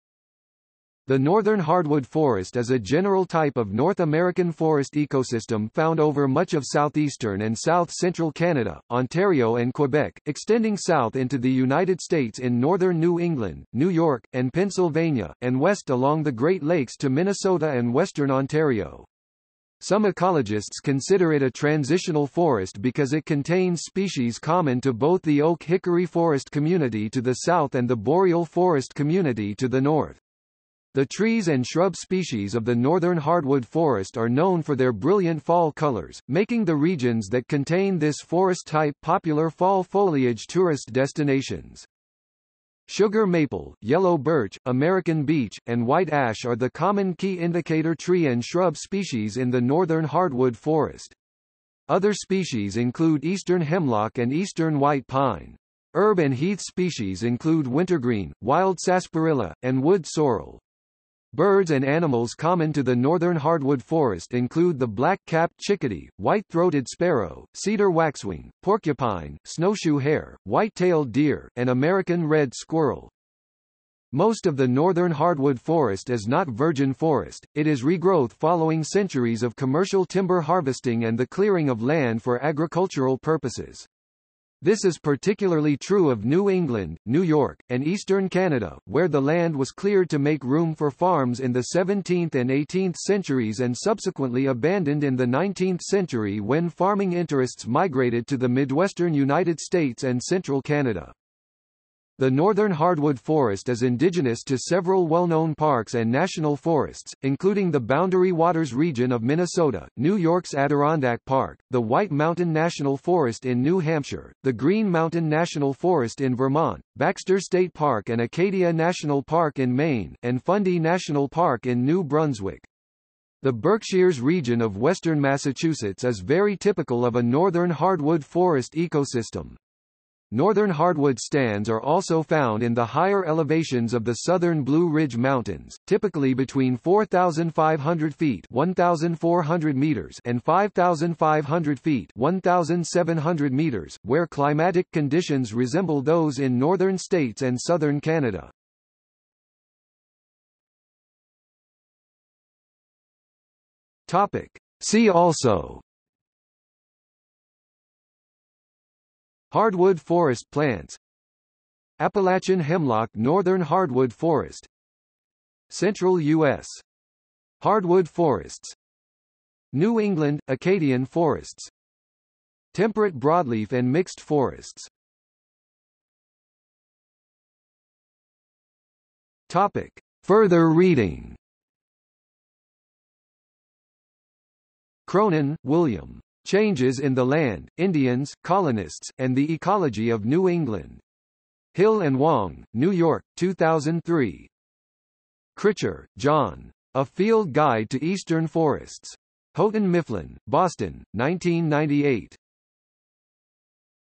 Northern hardwood forest | Wikipedia audio article
00:02:30 See also
00:02:55 Further reading

- Socrates

SUMMARY
The northern hardwood forest is a general type of North American forest ecosystem found over much of southeastern and south central Canada, Ontario and Quebec, extending south into the United States in northern New England, New York, and Pennsylvania, and west along the Great Lakes to Minnesota and western Ontario. Some ecologists consider it a transitional forest because it contains species common to both the oak-hickory forest community to the south and the Boreal forest community to the north. The trees and shrub species of the Northern Hardwood Forest are known for their brilliant fall colors, making the regions that contain this forest type popular fall foliage tourist destinations.
Sugar maple, yellow birch, American beech, and White Ash are the common key indicator tree and shrub species in the Northern Hardwood Forest. Other species include eastern hemlock and eastern white pine. Herb and heath species include wintergreen, wild sarsaparilla, and wood sorrel. Birds and animals common to the Northern Hardwood Forest include the black-capped chickadee, white-throated sparrow, cedar waxwing, porcupine, snowshoe hare, white-tailed deer, and American Red Squirrel.
Most of the Northern Hardwood Forest is not virgin forest, it is regrowth following centuries of commercial timber harvesting and the clearing of land for agricultural purposes. This is particularly true of New England, New York, and Eastern Canada, where the land was cleared to make room for farms in the 17th and 18th centuries and subsequently abandoned in the 19th century when farming interests migrated to the midwestern United States and central Canada.
The Northern Hardwood Forest is indigenous to several well-known parks and national forests, including the Boundary Waters region of Minnesota, New York's Adirondack Park, the White Mountain National Forest in New Hampshire, the Green Mountain National Forest in Vermont, Baxter State Park and Acadia National Park in Maine, and Fundy National Park in New Brunswick. The Berkshires region of western Massachusetts is very typical of a Northern Hardwood Forest ecosystem.  Northern hardwood stands are also found in the higher elevations of the southern Blue Ridge Mountains, typically between 4,500 feet (1,400 m) and 5,500 feet (1,700 m), where climatic conditions resemble those in northern states and southern Canada.